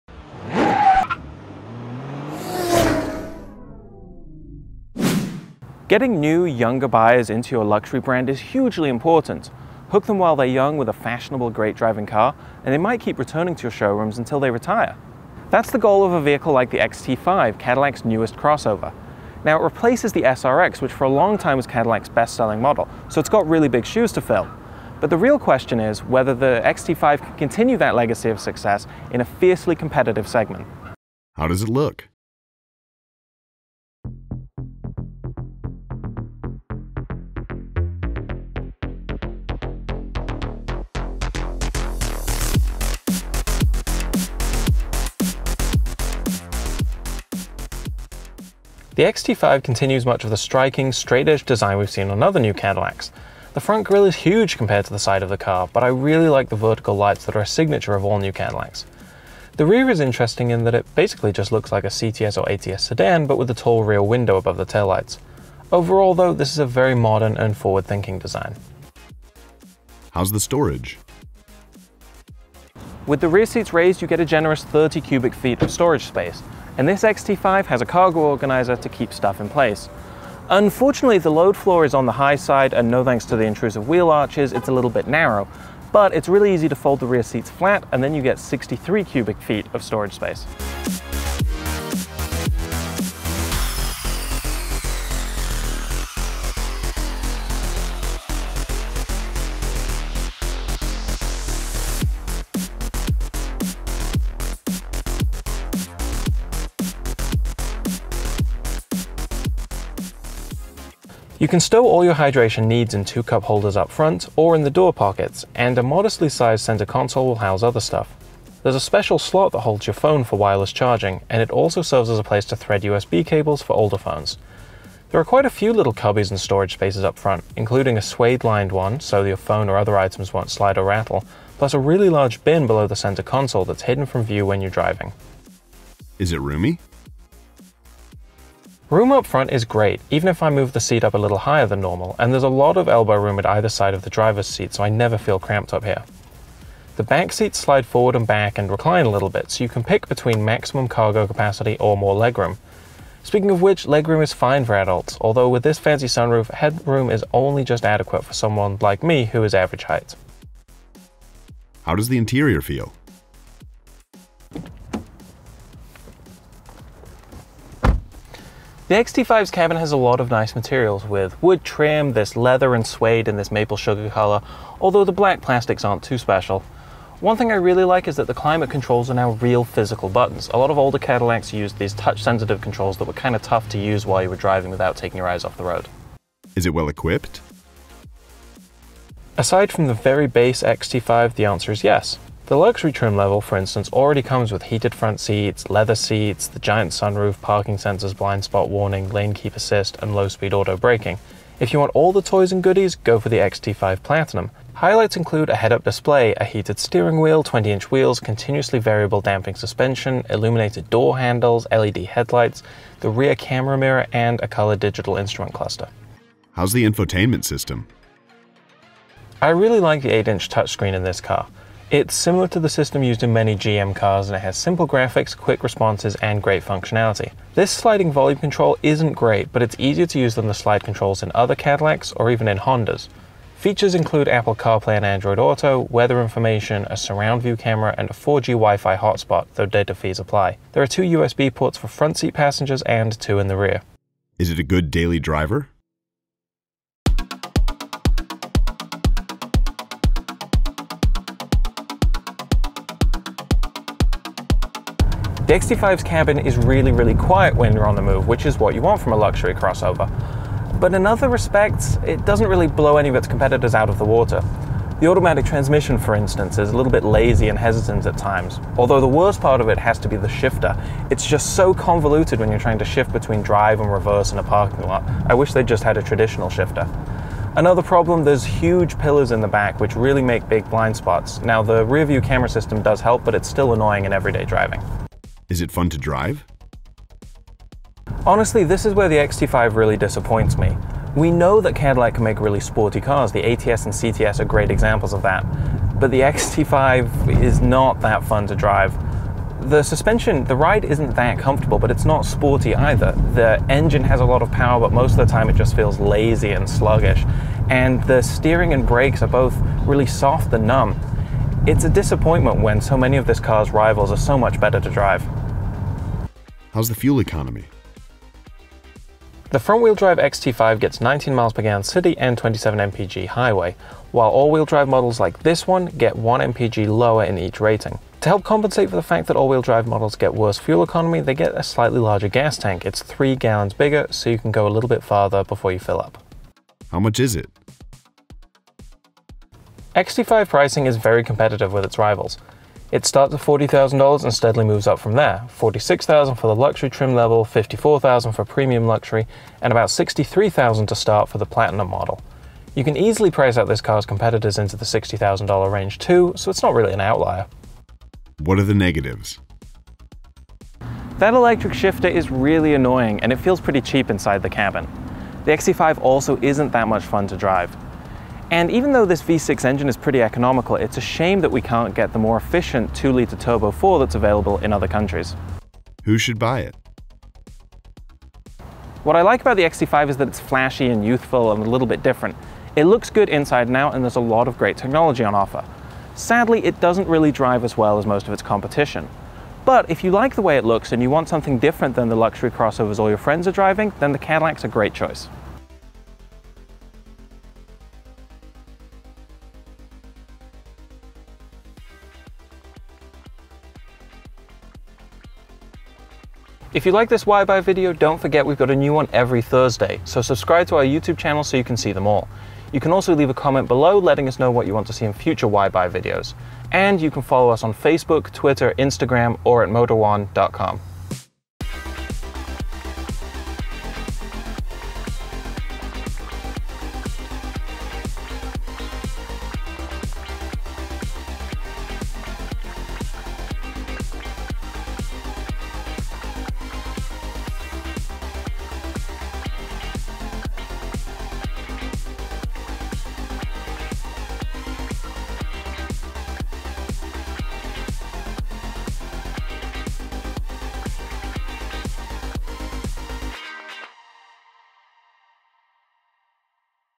Why Buy? | 2017 Cadillac XT5 Review
Motor1 Senior Editor Jake Holmes hops behind the wheel of Cadillac's stylish new midsize crossover the XT5, to find out if it has the chops to take on the rest of this hotly contested segment.

How does it look? 00:30
How's the storage? 01:09
Is it roomy? 02:16
How does the interior feel? 02:41
Is it well equipped? 03:06
How's the infotainment system? 03:36
Is it a good daily driver? 04:04
Is it fun to drive? 04:45
How's the fuel economy? 05:15
How much is it?  05:36
What are the negatives? 05:57
Who should buy it? 06:15

Crossovers tend to the be entry point to many luxury car brands, and Cadillac is no exception. The SRX was long the brand’s best-selling model, and so its replacement, the new Cadillac XT5, has big shoes to fill. It needs to pack enough style and refinement to convince buyers to move up from mainstream crossovers, and it must compete with the ever more competitive set of luxury crossovers from Japan and Europe.

The Cadillac XT5 looks sharp inside and out, and offers a truly impressive feature set. But does it drive well enough to stack up against the best new luxury crossovers? Find out in our latest Why Buy? video.

SPECIFICATIONS:
Engine: 3.6-Liter V6
Output: 310 Horsepower / 252 Pound-Feet
Transmission: 8-Speed Automatic
EPA Fuel Economy: 18 City / 26 Highway / 21 Combined

CREDITS
Host: Jake Holmes
DP: James Bradbury
Editor: James Bradbury